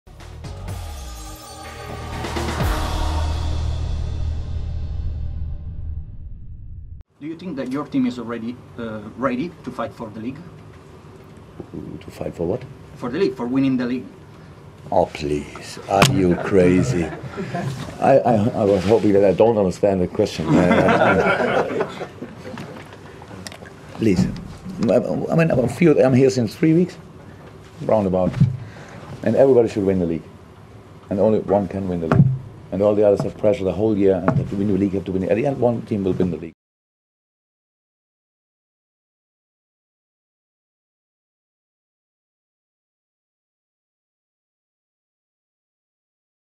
Jurgen Klopp's Amazing Reaction When Asked If Liverpool Can Win The League
Thanks